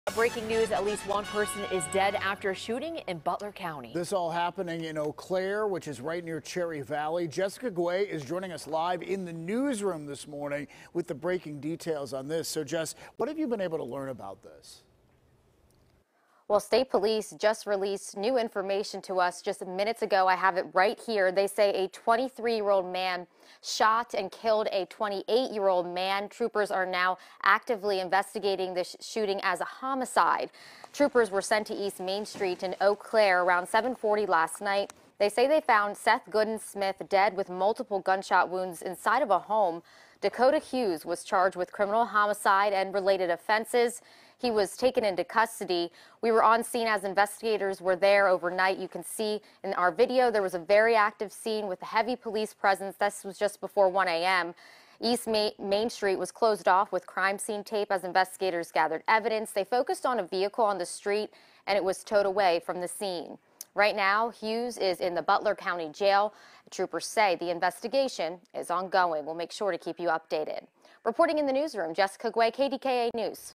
28-year-old man shot and killed in Eau Claire, Butler County
The shooting occurred just before 8 p.m. along East Main Street in Eau Claire near Cherry Valley.